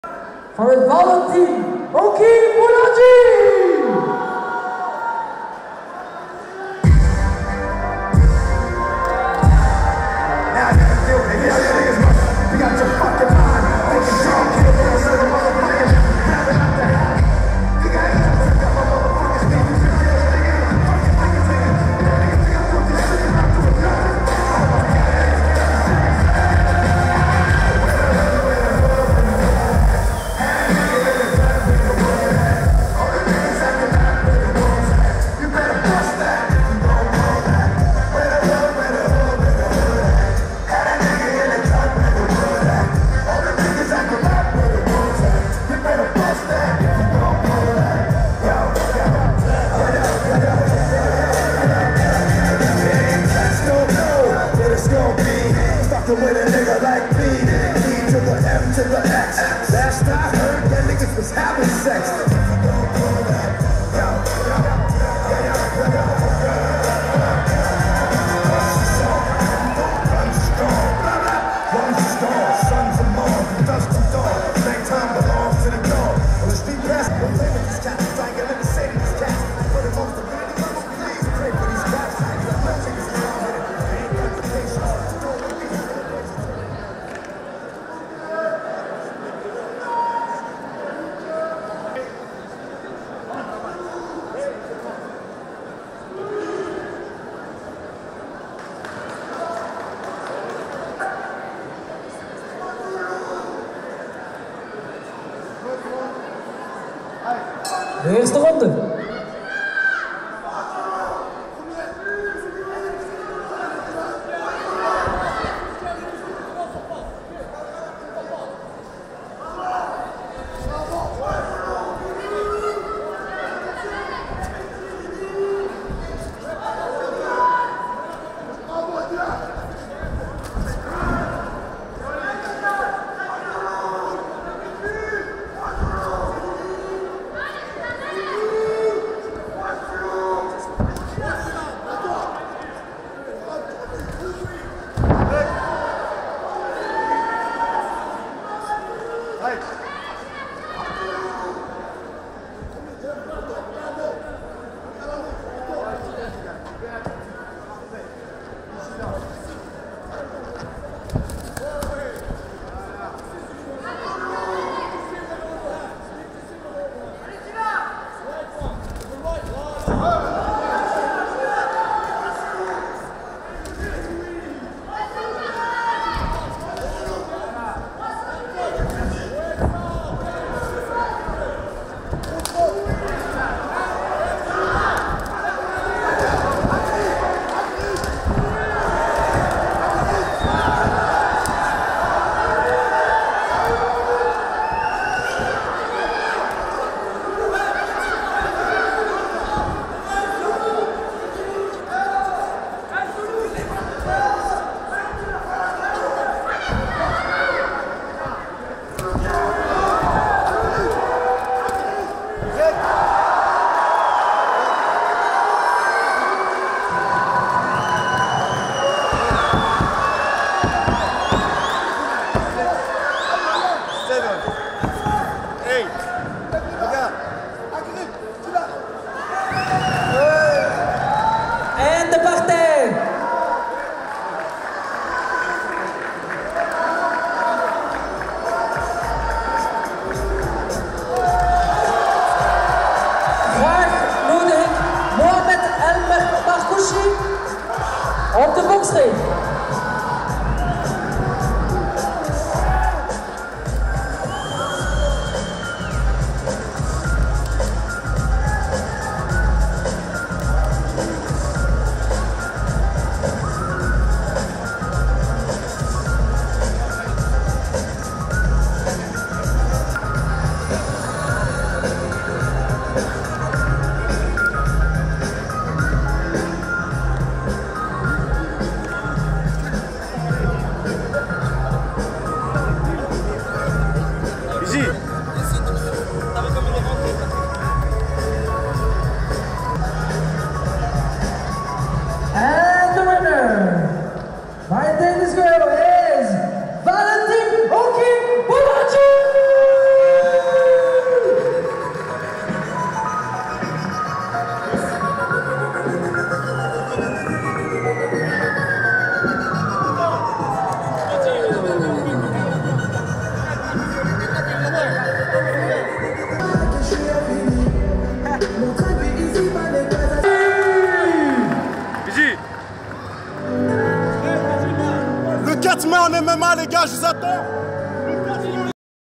Crazy victory in K1 BY KO in the 1st round by Bolaji Oki (HD) - Ronse 23.03.2019
Crazy victory in K1 by KO in the first round by Bolaji Oki aka the Zulu Warrior in Ronse (Belgium) on Saturday 23 March 2019.
Bolaji Oki is a MMA fighter fighting for the Valon Team - Da Vinci Fighting in  Brussels (Belgium).